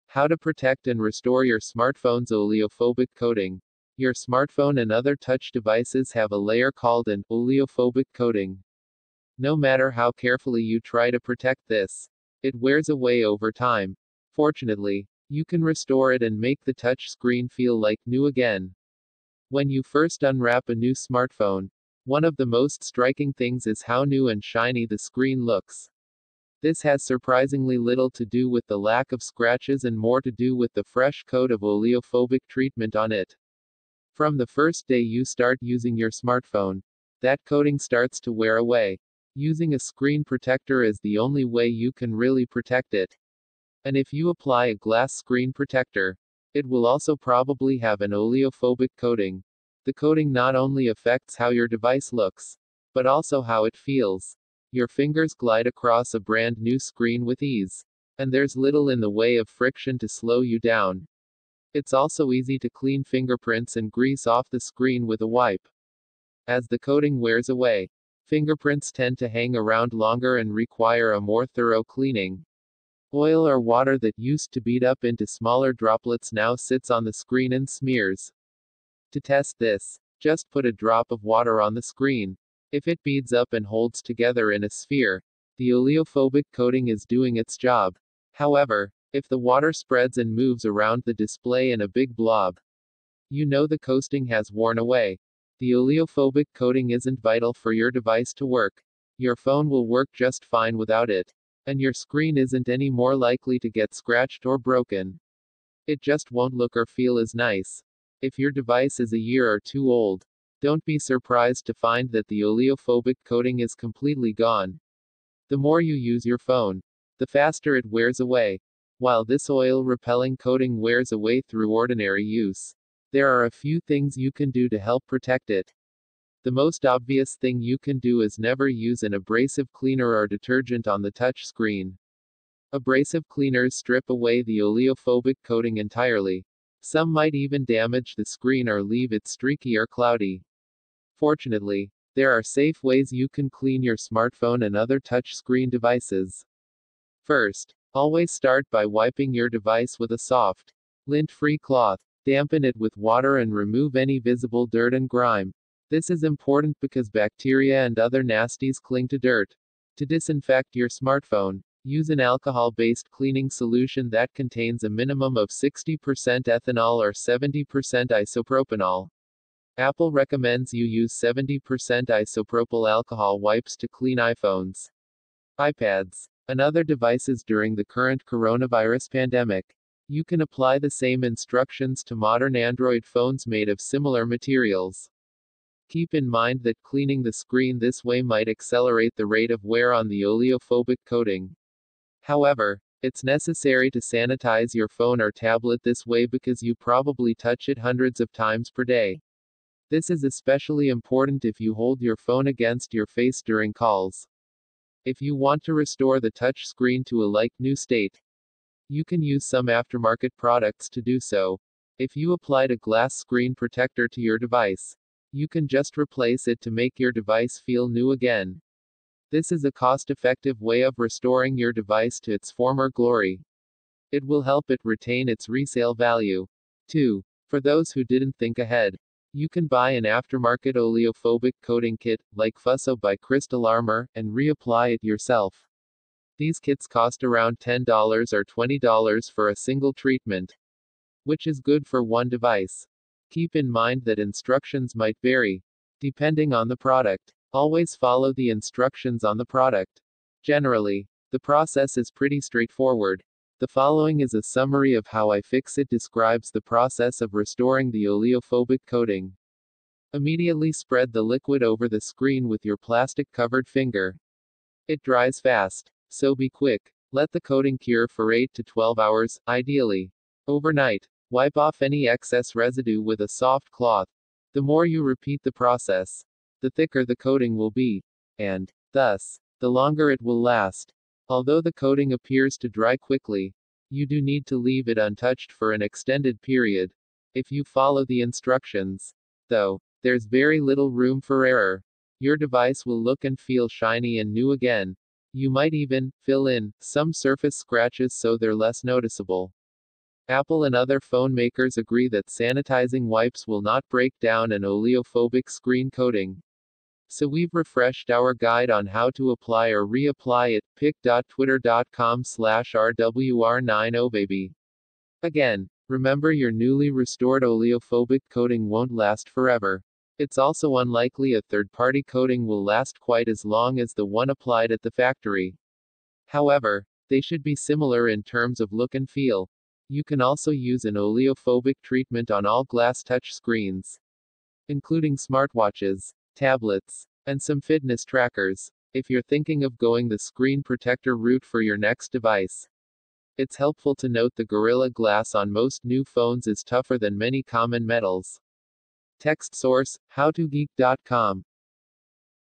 How to Protect and Restore Your Smartphone’s Oleophobic Coating
Your smartphone and other touch devices have a layer called an “oleophobic coating.” No matter how carefully you try to protect this, it wears away over time. Fortunately, you can restore it and make the touch screen feel like new again.

When you first unwrap a new smartphone, one of the most striking things is how new and shiny the screen looks. This has surprisingly little to do with the lack of scratches and more to do with the fresh coat of oleophobic treatment on it.

From the first day you start using your smartphone, that coating starts to wear away. Using a screen protector is the only way you can really protect it. And if you apply a glass screen protector, it will also probably have an oleophobic coating.

The coating not only affects how your device looks, but also how it feels. Your fingers glide across a brand-new screen with ease, and there’s little in the way of friction to slow you down. It’s also easy to clean fingerprints and grease off the screen with a wipe.

As the coating wears away, fingerprints tend to hang around longer and require a more thorough cleaning. Oil or water that used to bead up into smaller droplets now sits on the screen and smears.

To test this, just put a drop of water on the screen. If it beads up and holds together in a sphere, the oleophobic coating is doing its job. However, if the water spreads and moves around the display in a big blob, you know the coasting has worn away.

The oleophobic coating isn’t vital for your device to work. Your phone will work just fine without it, and your screen isn’t any more likely to get scratched or broken. It just won’t look or feel as nice.

If your device is a year or two old, don’t be surprised to find that the oleophobic coating is completely gone. The more you use your phone, the faster it wears away.

While this oil-repelling coating wears away through ordinary use, there are a few things you can do to help protect it. The most obvious thing you can do is never use an abrasive cleaner or detergent on the touch screen.

Abrasive cleaners strip away the oleophobic coating entirely. Some might even damage the screen or leave it streaky or cloudy. Fortunately, there are safe ways you can clean your smartphone and other touch screen devices.

First, always start by wiping your device with a soft, lint-free cloth. Dampen it with water and remove any visible dirt and grime. This is important because bacteria and other nasties cling to dirt.

To disinfect your smartphone, use an alcohol-based cleaning solution that contains a minimum of 60 percent ethanol or 70 percent isopropanol. Apple recommends you use 70 percent isopropyl alcohol wipes to clean iPhones, iPads, and other devices during the current coronavirus pandemic. You can apply the same instructions to modern Android phones made of similar materials.

Keep in mind that cleaning the screen this way might accelerate the rate of wear on the oleophobic coating. However, it’s necessary to sanitize your phone or tablet this way because you probably touch it hundreds of times per day. This is especially important if you hold your phone against your face during calls.

If you want to restore the touch screen to a like-new state, you can use some aftermarket products to do so.

If you applied a glass screen protector to your device, you can just replace it to make your device feel new again. This is a cost-effective way of restoring your device to its former glory. It will help it retain its resale value, too.

For those who didn’t think ahead, you can buy an aftermarket oleophobic coating kit (like Fusso by Crystal Armor) and reapply it yourself. These kits cost around $10 or $20 for a single treatment, which is good for one device.

Keep in mind that instructions might vary, depending on the product. Always follow the instructions on the product.

Generally, the process is pretty straightforward. The following is a summary of how iFixit describes the process of restoring the oleophobic coating:

Immediately spread the liquid over the screen with your plastic-covered finger. (It dries fast, so be quick!)

Let the coating cure for eight to 12 hours (ideally, overnight). Wipe off any excess residue with a soft cloth.

The more you repeat the process, the thicker the coating will be, and, thus, the longer it will last. Although the coating appears to dry quickly, you do need to leave it untouched for an extended period.

If you follow the instructions, though, there’s very little room for error. Your device will look and feel shiny and new again. You might even “fill in” some surface scratches so they’re less noticeable.

Apple and other phone makers agree that sanitizing wipes will *not* break down an oleophobic screen coating. So we've refreshed our guide on how to apply or re-apply it

Ag…